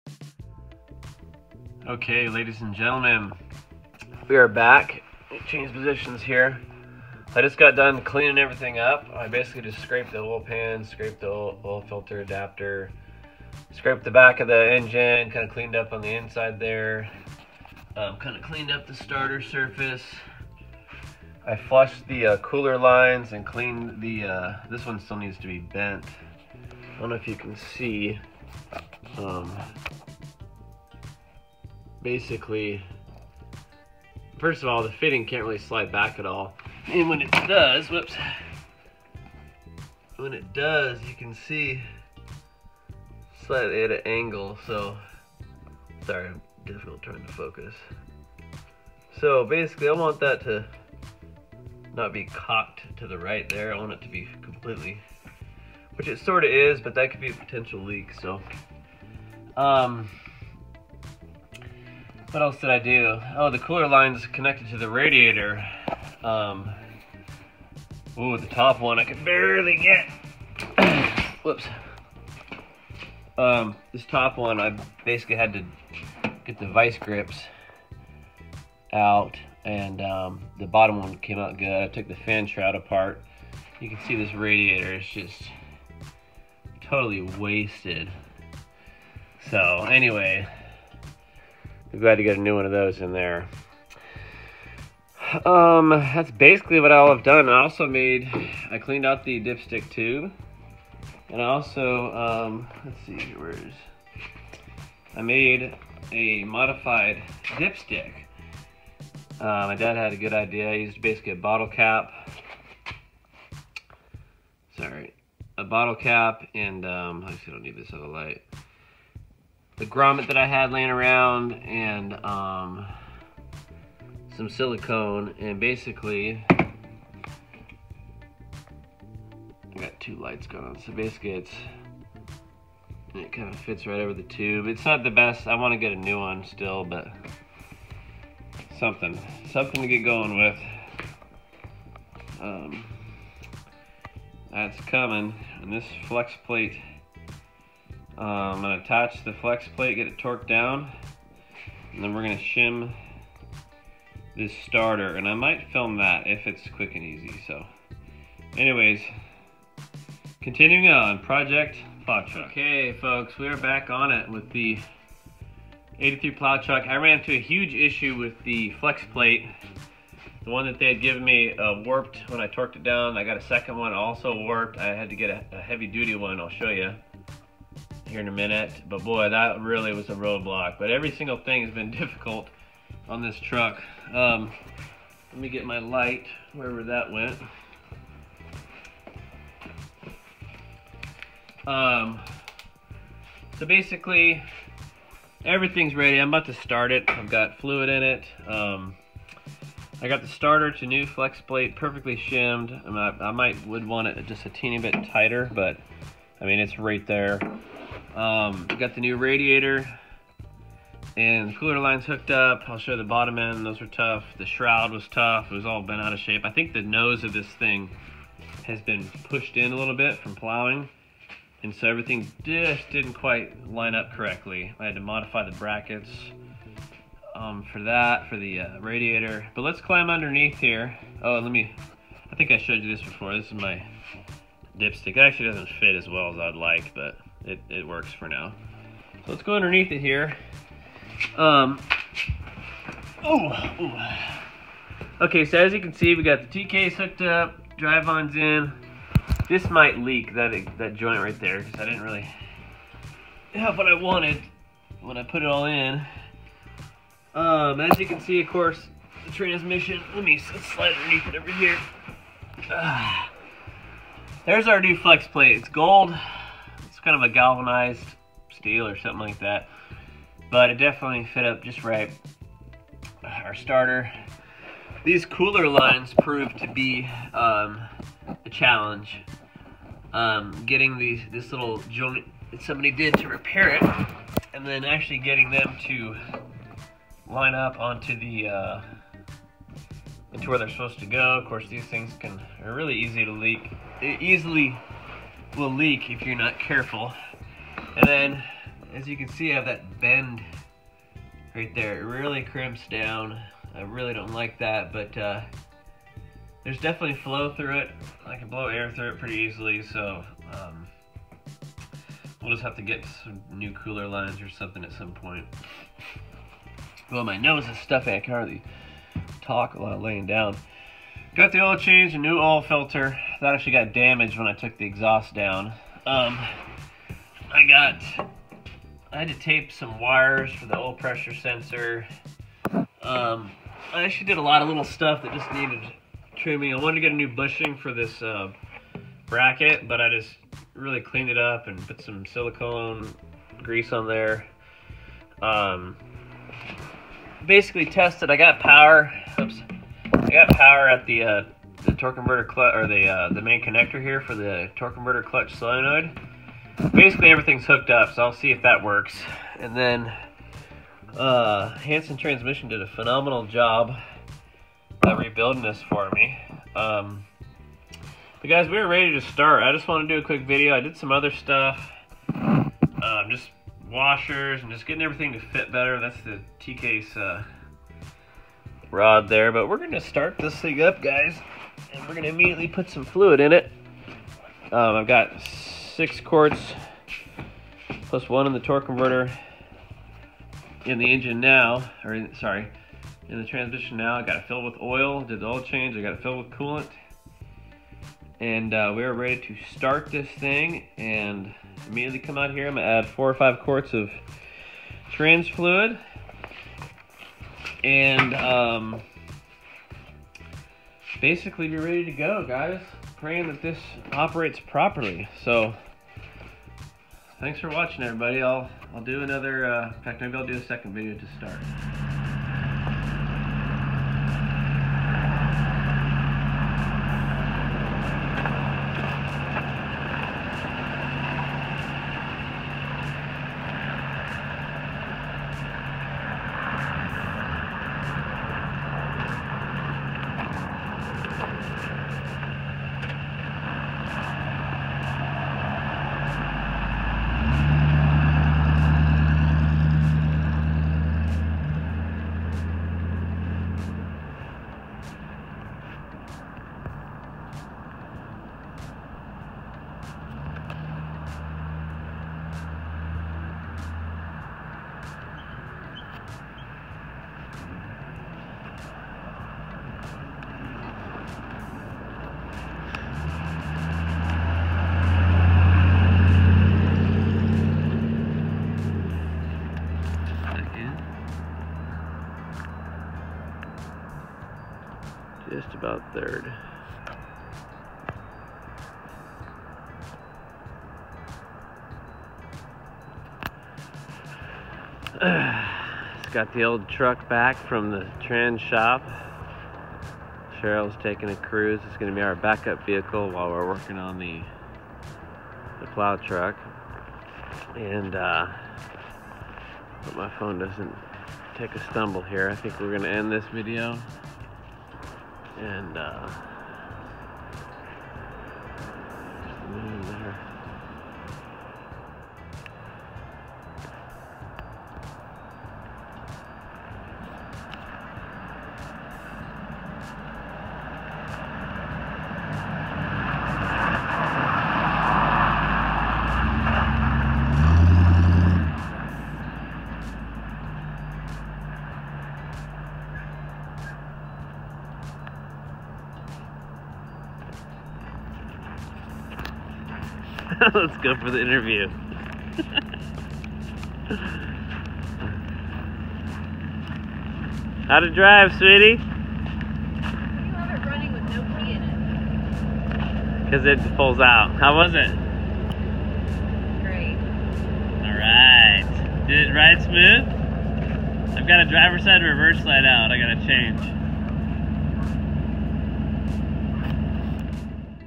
Ep.30 1983 Chevrolet K10 700R4 Pt.4 Install and Test Drive!
In this episode the Plow Truck gets closer to running with the install of the 700R4 automatic transmission. This is three videos edited together and what I didn’t record was the big challenge I had with two different flexplates that I had for it. Both kept warping when torqued down, luckily one of the parts stores in town carried a Heavy Duty version and everything fit up perfect! There were several more challenges, shimming the starter, the exhaust was bent, basically each part of the install was giving me a headache lol.